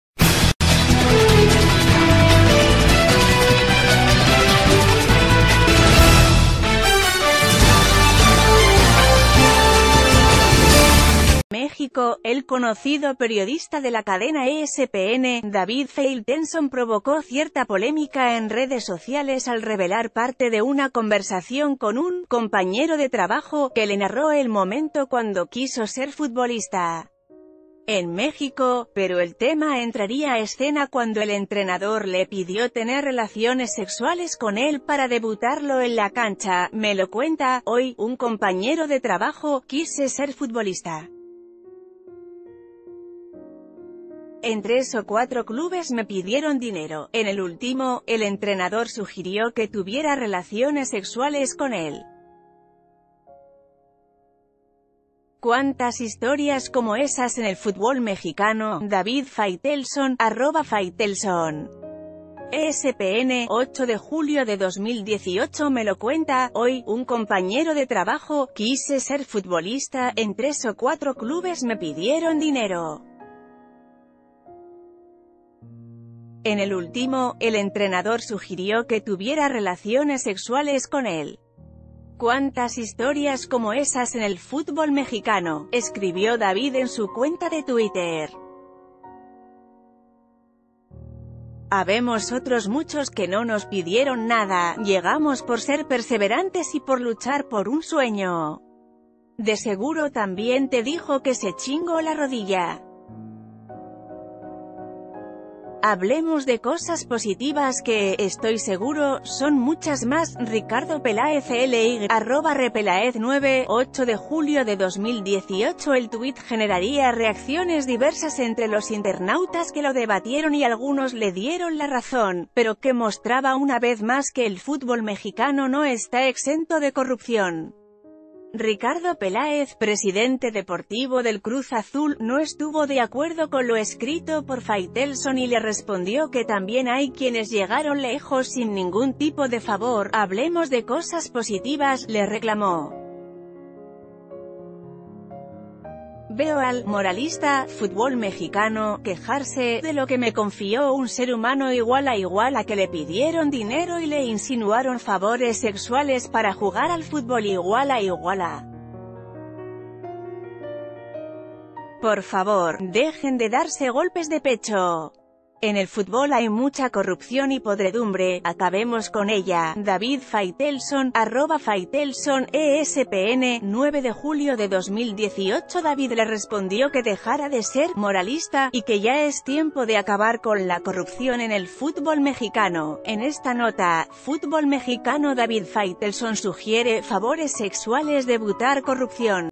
Faitelson sugiere que se piden favores sexuales para debutar
México.-El conocido periodista de la cadena ESPN, David Failtenson provocó cierta polémica en redes sociales al revelar parte de una conversación con un “compañero de trabajo” que le narró el momento cuando quiso ser futbolista en México. Pero el tema entraría a escena cuando el entrenador le pidió ...